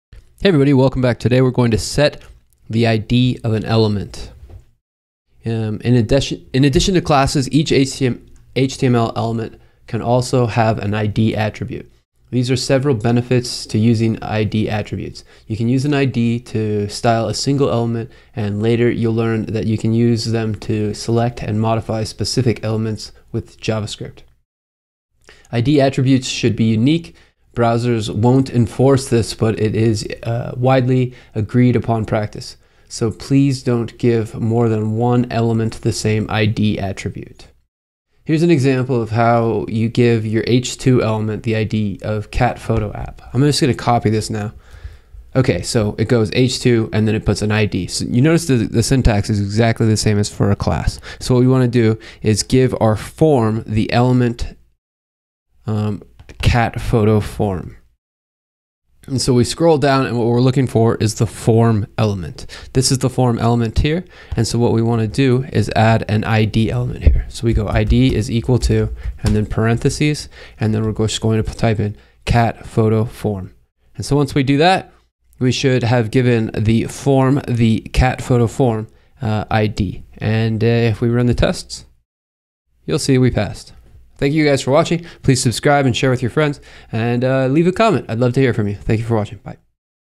Basic CSS: Set the id of an Element | freeCodeCamp
Today we have a basic css tutorial on how to set the id of an element. This is very basic CSS.